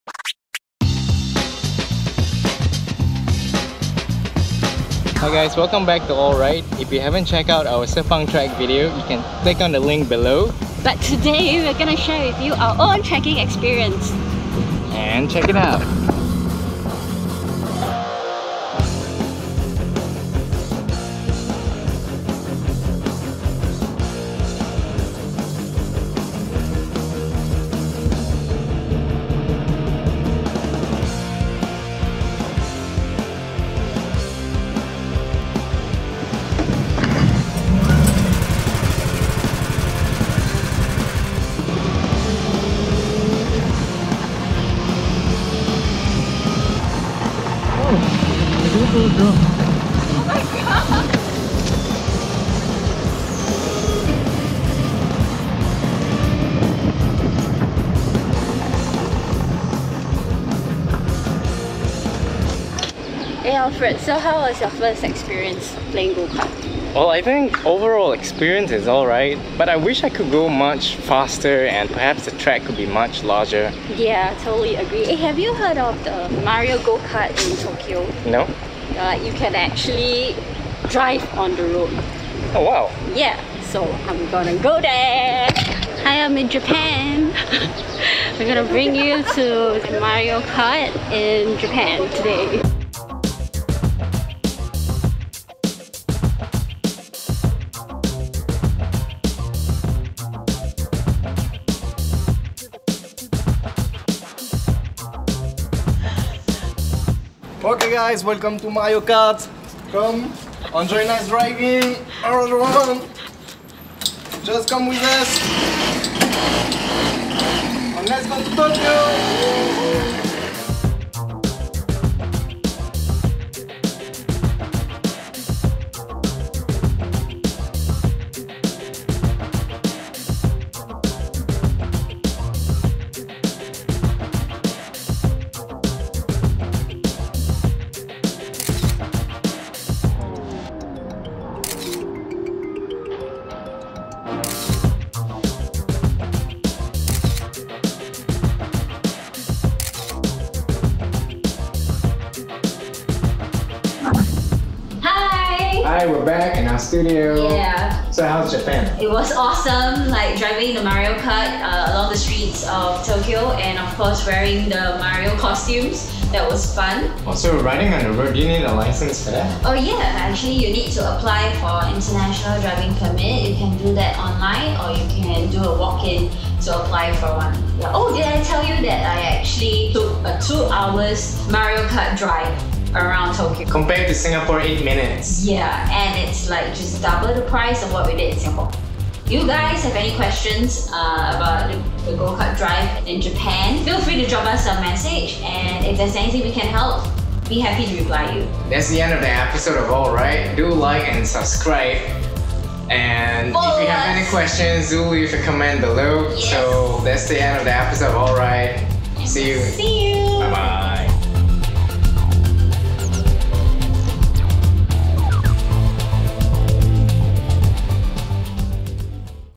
AWERIDE GOKART
SELAMAT HARI RAYA PUASA!
Looking for some fun activities with your family this long weekend?
Let Lynn and Alfred show you what its like to go karting!